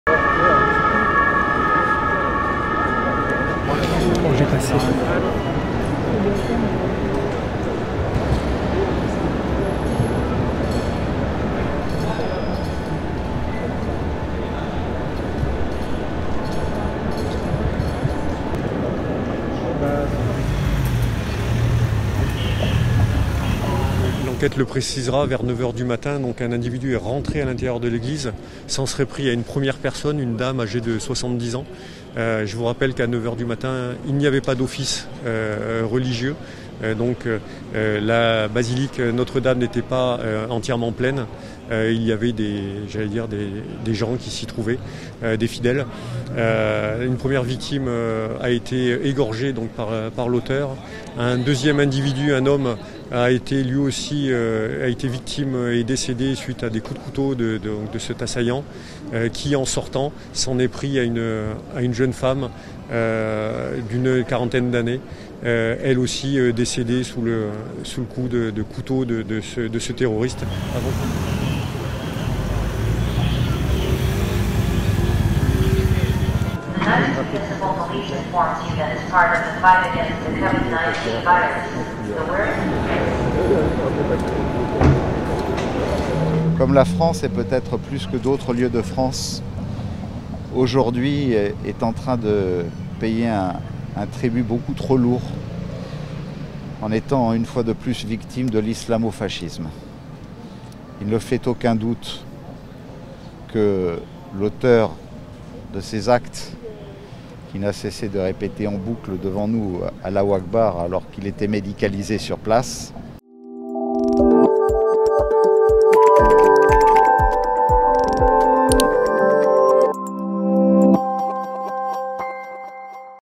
Tres muertos en un ataque con cuchillo en una iglesia de Niza, en el sudoeste de Francia
Tres personas murieron, al menos una de ellas degollada, y varias resultaron heridas hoy en un ataque con cuchillo en una iglesia en pleno corazón de Niza, en el sureste de Francia, otra vez sacudida por el extremismo tras la decapitación de un profesor a manos de un islamista hace dos semanas.